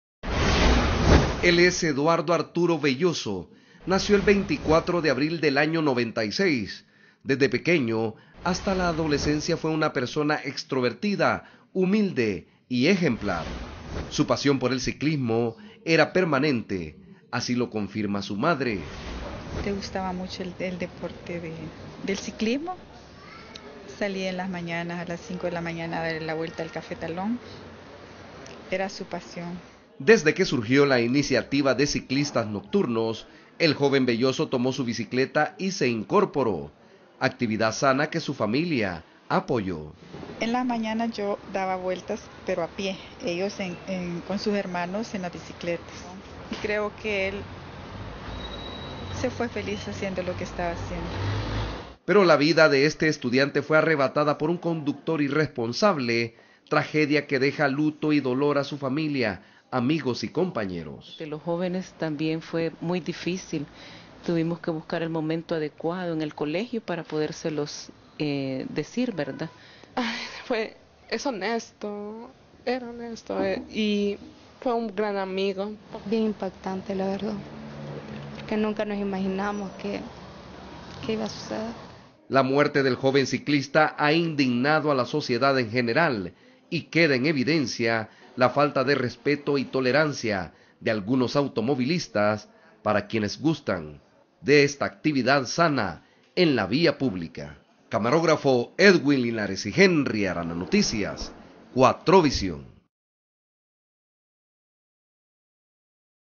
Luto e indignación
Familiares y amigos de Eduardo Belloso de 17 años, ciclista que murió atropellado, lo recuerdan como un joven alegre con una gran pasión por el ciclismo. (21/09/2013)